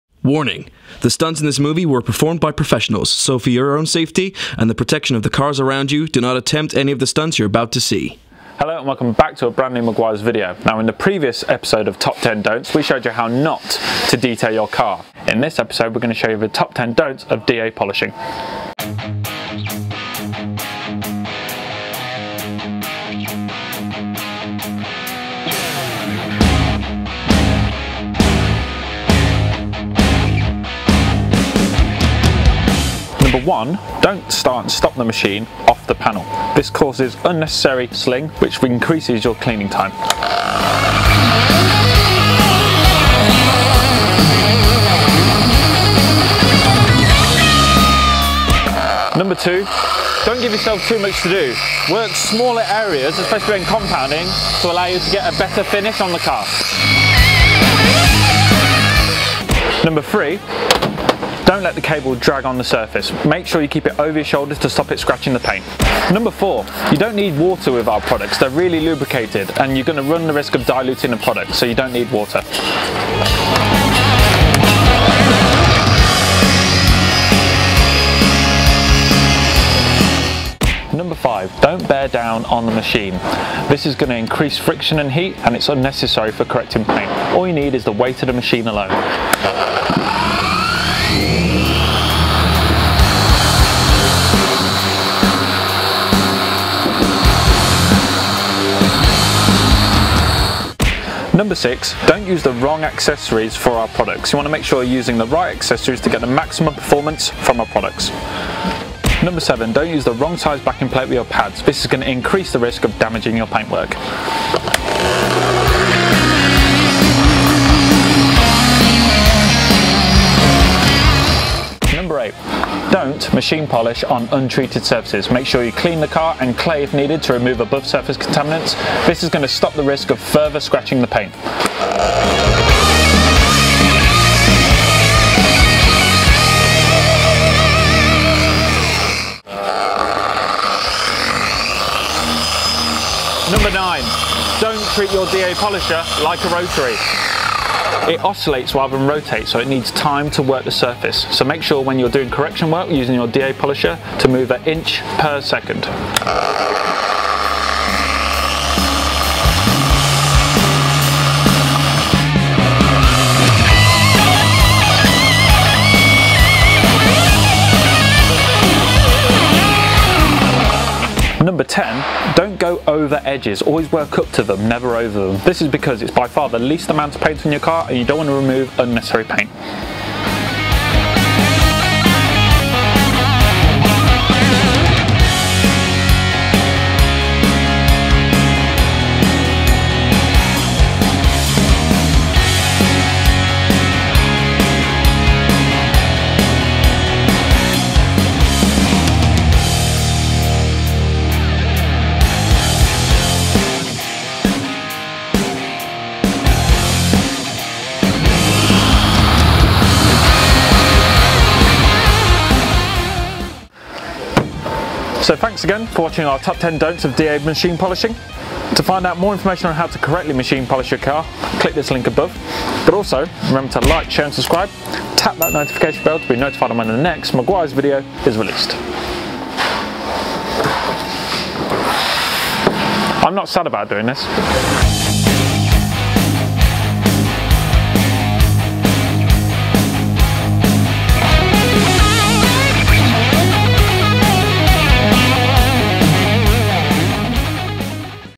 Top 10 DONT'S of DA Machine Polishing
Welcome to the Meguiar's UK YouTube Channel

The last episode of Top 10 DONT'S covered the day to day car care processes and what not to do. In today's video we share the Top 10 DONT'S of DA Machine Polishing.